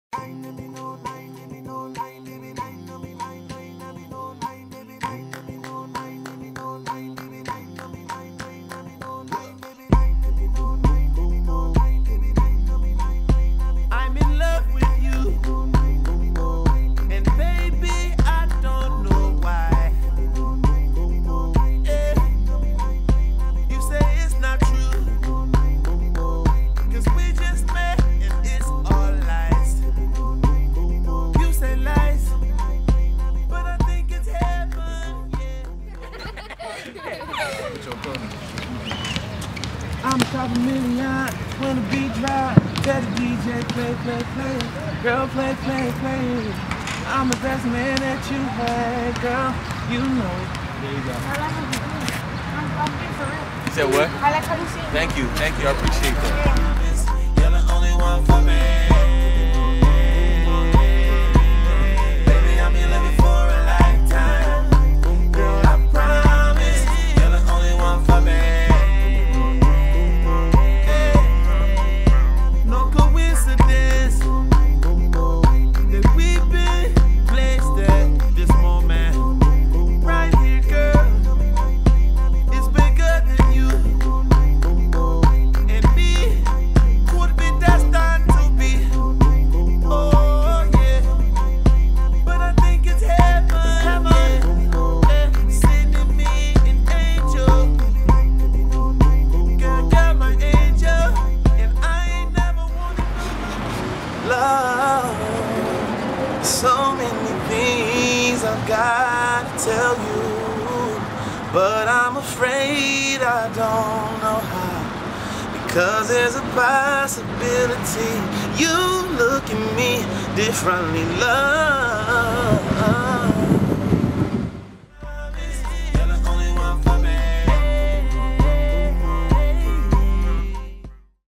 KEITH STEWART Photoshoot w/ MOE CHEEZ
A day and a life of Keith Stewart! Music and new visuals coming soon!